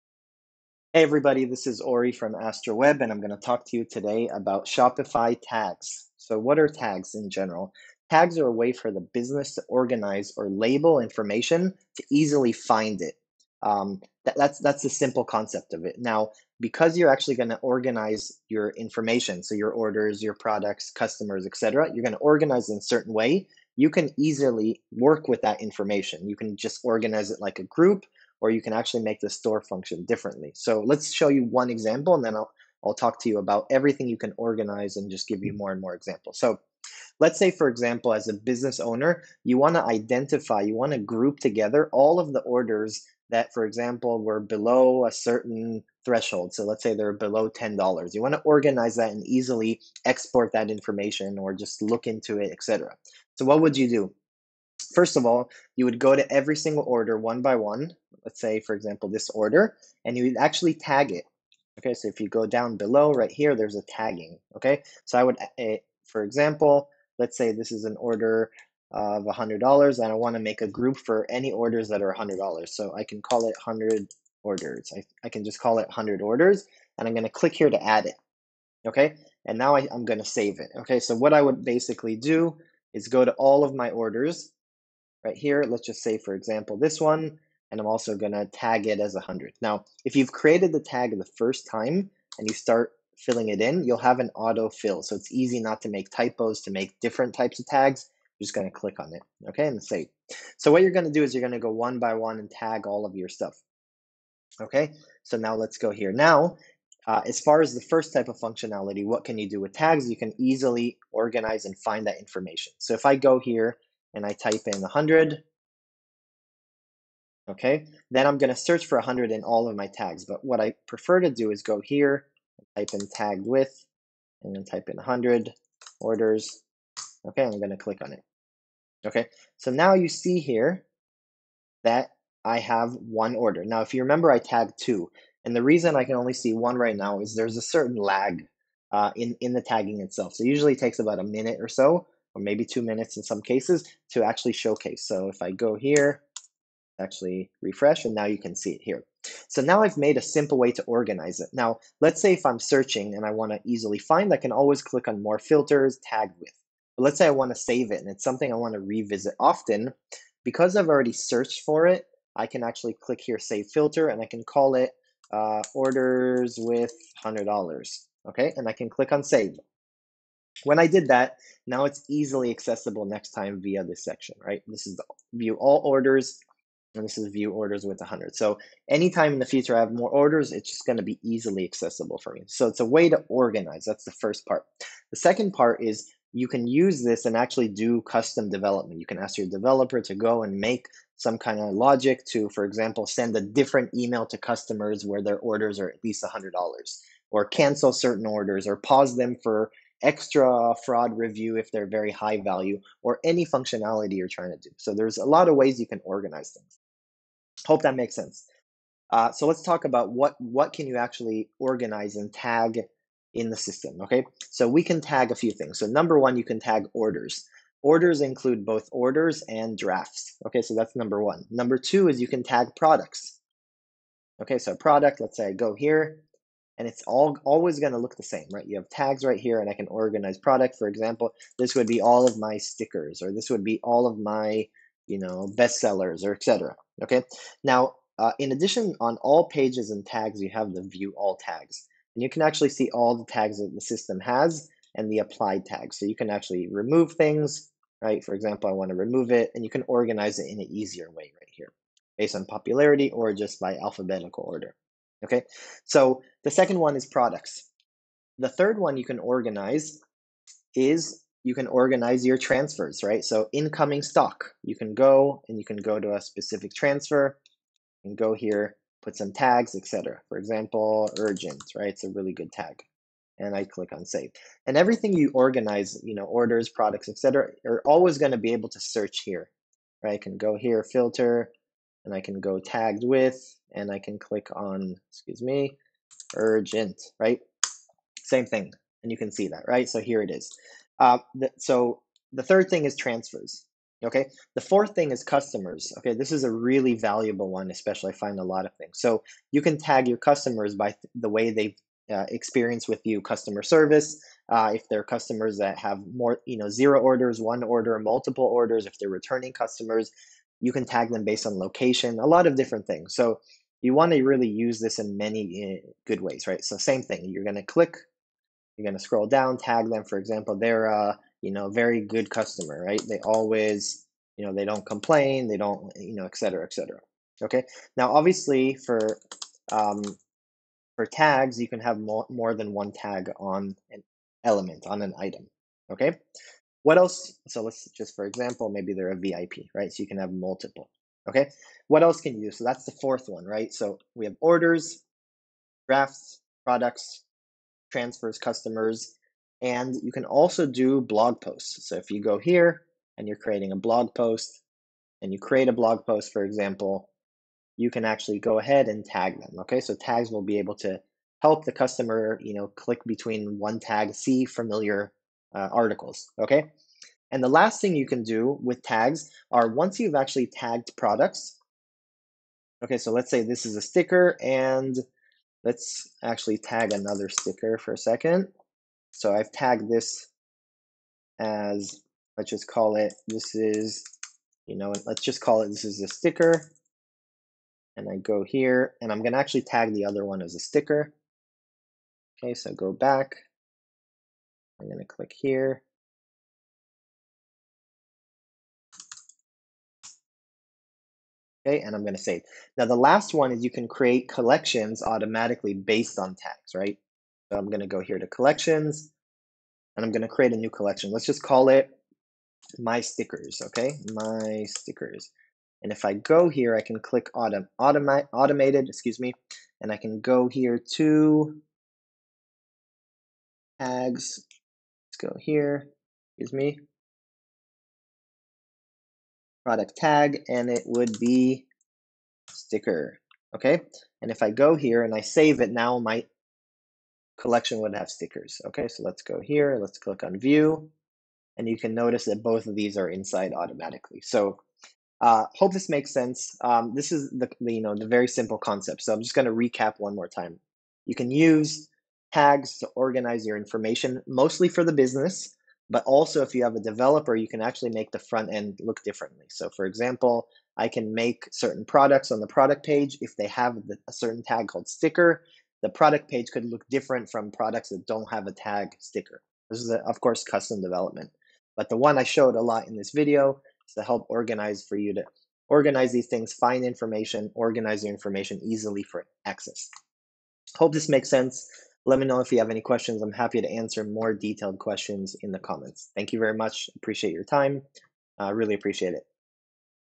Shopify Tags Concept and Walkthrough
This video explains what tags are and how you use them. We include discussion about how to organize and label your data as well as an example of how your customer experience a different page based on tags.

Tags can be added to:
- Orders (regular orders and drafts)
- Products
- Automate a Collection
- Customers
- Blog Posts